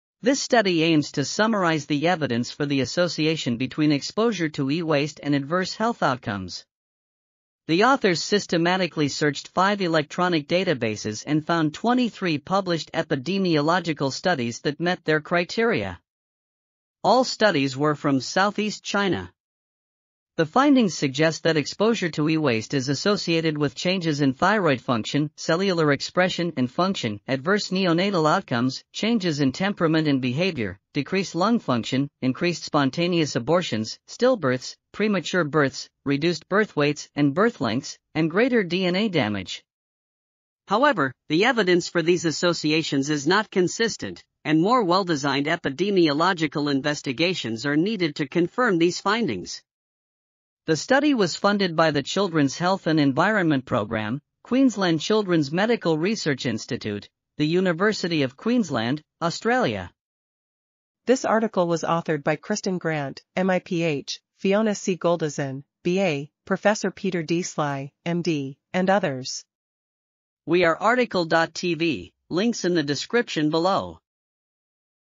Health consequences of exposure to e-waste: a systematic review | RTCL.TV
### Keywords ###

### Article Attribution ###
Title: Health consequences of exposure to e-waste: a systematic review
Authors: Kristen Grant, MIPH, Fiona C Goldizen, BA, Prof. Peter D Sly, MD, Marie-Noel Brune, MSc, Maria Neira, MD, Prof. Martin van den Berg, PhD ,and Rosana E Norman, PhD
Publisher: Elsevier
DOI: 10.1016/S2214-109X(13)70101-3

### Video Timestamps ###
0:00:00 - Summary
0:01:00 - Title
0:01:10 - Tail
0:01:14 - End